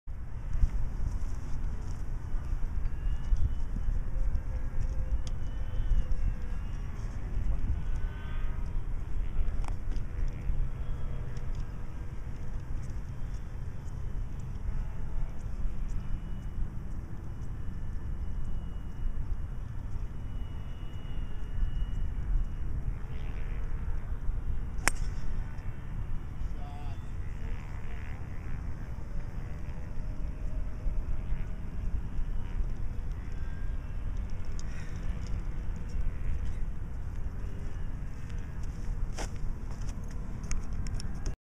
Hitting a 15* Honma 3 Wood draw from 235 yards in Encino CA, Los Angeles
Hitting a 15* Honma 3 Wood draw from 235 yards draw. Love this club.

First posted video...please rate nicely :)